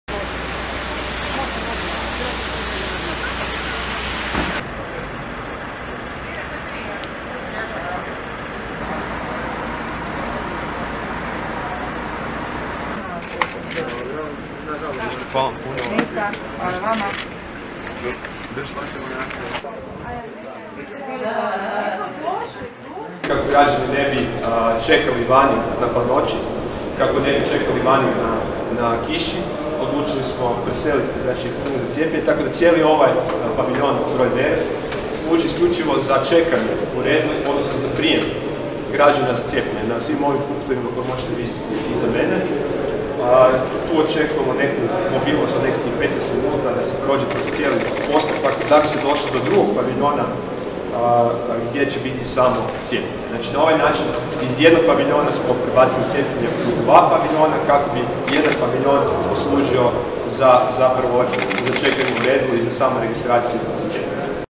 Gradonačelnik Tomašević obišao Drive in punkt za testiranje i novi Cijepni punkt na Velesajmu
Zagreb, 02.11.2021 - Gradonačelnik Tomašević obišao Drive in punkt za testiranje i novi Cijepni punkt na Velesajmu. Snimio: Marko Todorov / CROPIX